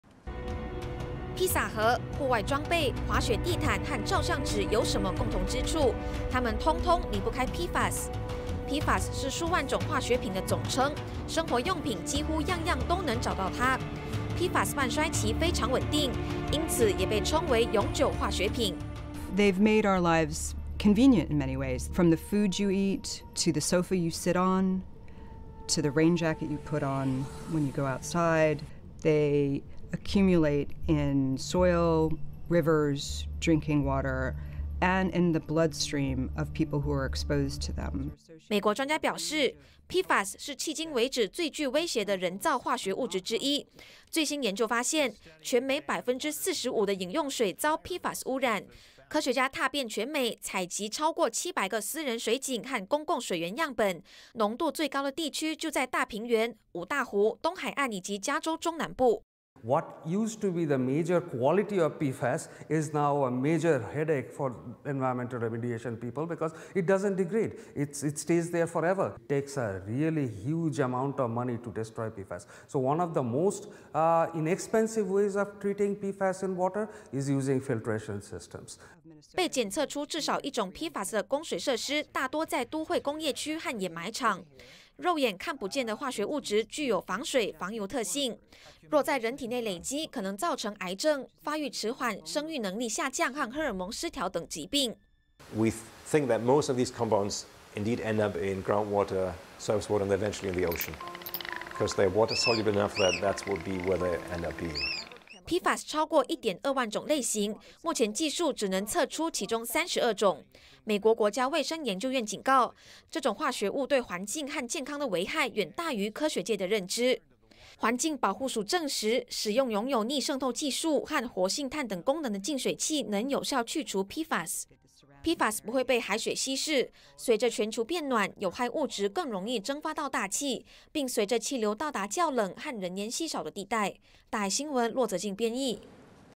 PFAS進入美國供水系統 近五成飲用水遭汙染
您聽過PFAS嗎？也許這個名詞對多數人來說不熟悉，但家裡的日常用品絕對看得見它的蹤跡。PFAS擁有防水防油的特性，常被用作表面塗料，例如地毯、不沾鍋、雨衣、食品包裝等，但其實PFAS在環境或人體內都難以分解。最新研究發現，美國將近一半的飲用水不安全，受到PFAS汙染。科學家指出，若在人體內累積，可能造成癌症、高膽固醇、肝損傷、生育能力下降等問題。

披薩盒、戶外裝備、滑雪地毯、照相紙，有什麼共通之處？它們通通離不開PFAS。PFAS是數萬種化學品的總稱，生活用品幾乎樣樣都能找到它，PFAS半衰期非常穩定，因此也被稱為「永久化學品」。

彭博社資深記者 貝克：「(PFAS)為我們的生活帶來很多便利，從你吃的食物，到你坐的沙發，再到外出時穿的雨衣(等等)，(PFAS)堆積在土壤、河流、飲用水，以及接觸到它們的人的血液中。」

美國專家表示，PFAS是迄今為止最具威脅性的人造化學物質之一。最新研究發現，全美45%的飲用水遭PFAS汙染。科學家踏遍全美，採集超過700個私人水井和公共水源樣本，濃度最高的地區就在大平原、五大湖、東海岸，以及加州中南部。

環境工程教授 薩爾卡：「曾經保證PFAS質量的優點，如今卻成為環境一大頭痛問題，因為它不會降解，會永遠留在那，投入巨額資金才能銷毀PFAS，想解決飲用水中的PFAS，最便宜的方法就是使用濾水器。」

被檢測出至少一種PFAS的供水設施，大多在都會工業區和掩埋場。肉眼看不見的化學物質，具有防水防油特質，若在人體內累積，可能造成癌症、發育遲緩、生育能力下降和荷爾蒙失調等疾病。

海洋學教授 羅曼：「我們認為這種化合物大部分最後都會流入地下水系統，最終進入海洋，它們具有水溶性特質，這將是最終的結果。」

PFAS超過1.2萬種類型，目前技術只能測出其中32種。美國國家衛生研究院警告，這種化合物對環境和健康的危害，遠大於科學界的認知。環境保護署證實，使用擁有逆滲透技術和活性炭等功能的淨水器，能有效去除PFAS。PFAS不會被海水稀釋，隨著全球變暖，有害物質更容易蒸發到大氣，並跟隨氣流到達較冷和人煙稀少的地帶。

APTN：駱澤婧 編譯 謝雅嵐 剪輯